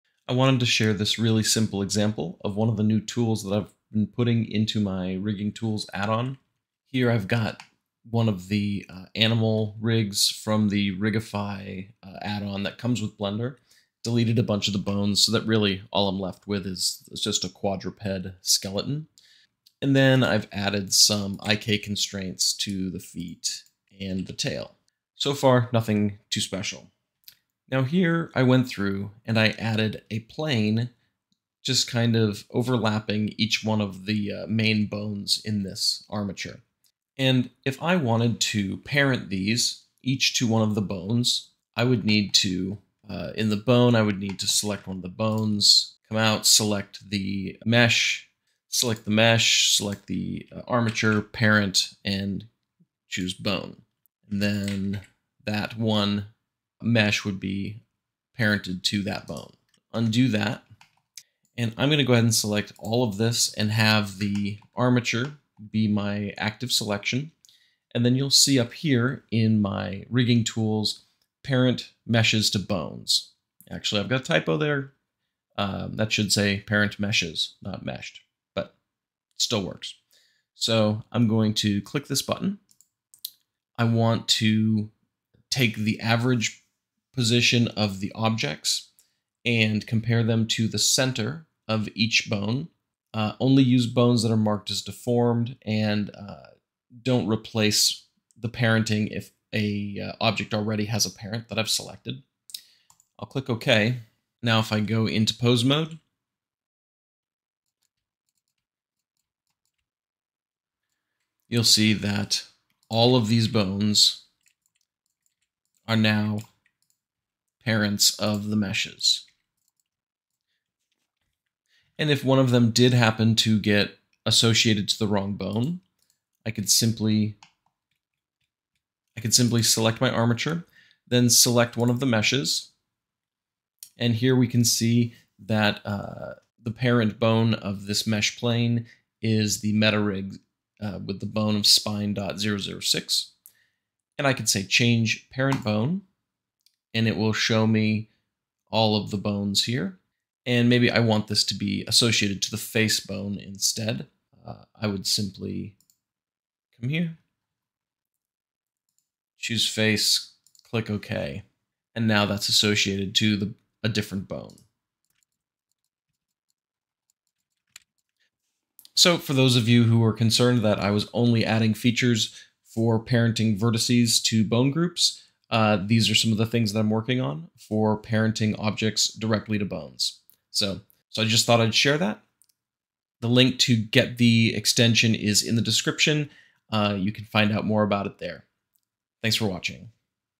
I Was Tired of Tedious Rigging – So I Built This Extension for You!
In this quick Blender tutorial, I demonstrate a powerful new feature in my custom rigging tools add-on: automatic parenting of multiple mesh objects to bones in an armature with a single click. Using a simplified quadruped rig from Rigify, I show how this tool streamlines the process of assigning meshes to deform bones based on spatial proximity—saving time and reducing manual work. Perfect for animators, riggers, and digital artists looking to improve workflow efficiency in Blender 3D. Whether you're rigging characters or building tools, this feature can help level up your Blender rigging game.